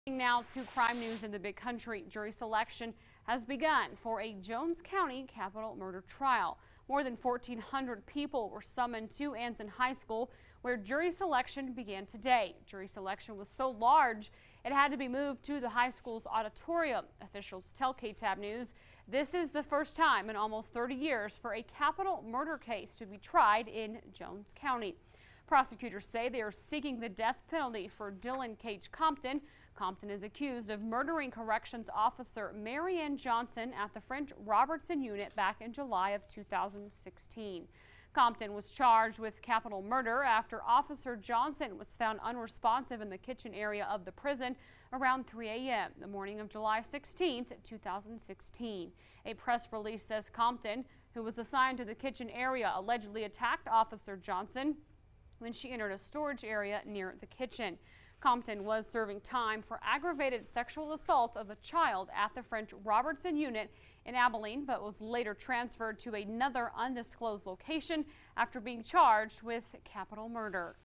1,400+ potential jurors summoned to Jones County Capital Murder trial, death penalty sought
1,400+ potential jurors summoned to Jones County Capital Murder trial, death penalty sought